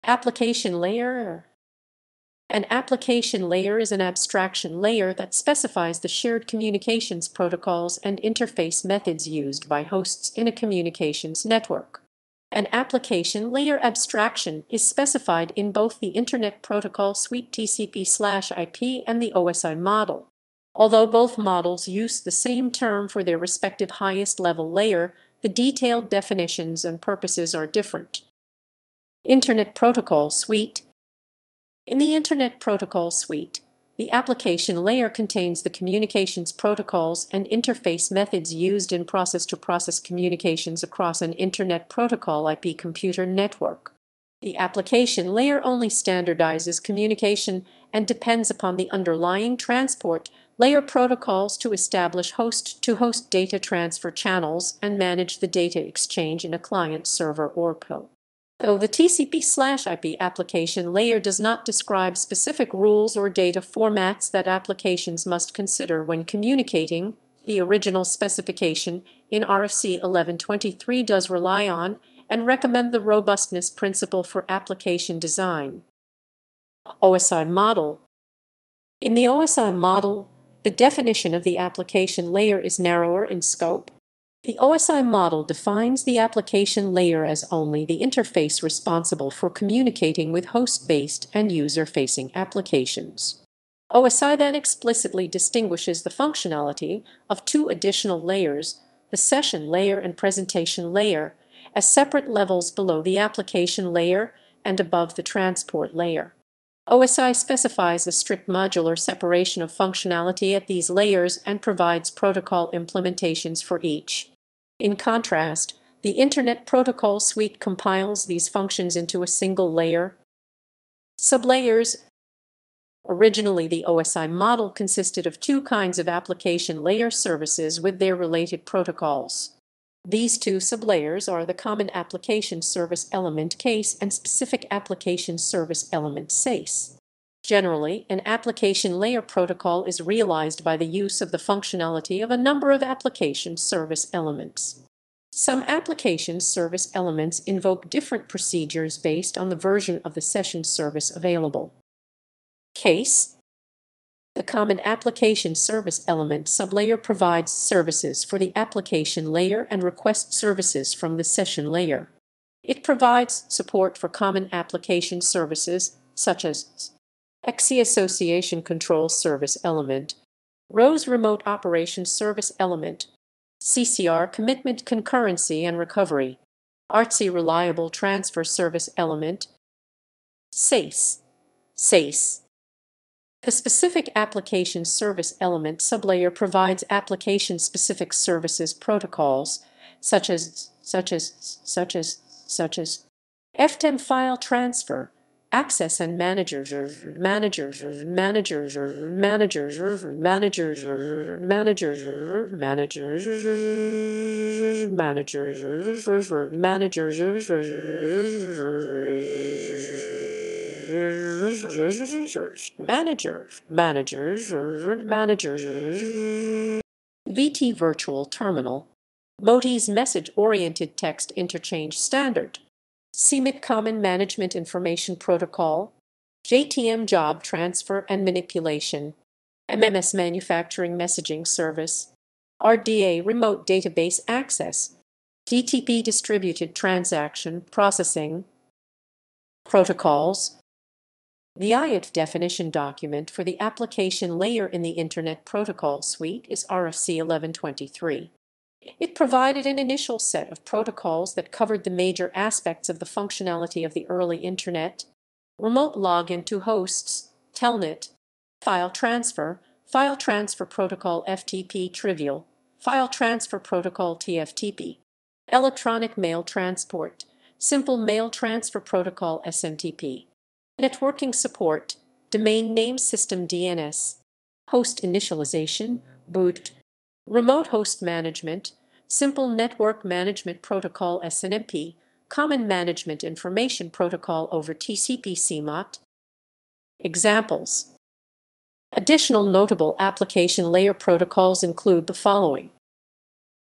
Application layer
An application layer is an abstraction layer that specifies the shared communications protocols and interface methods used by hosts in a communications network. An application layer abstraction is specified in both the Internet Protocol Suite (TCP/IP) and the OSI model. Although both models use the same term for their respective highest-level layer, the detailed definitions and purposes are different.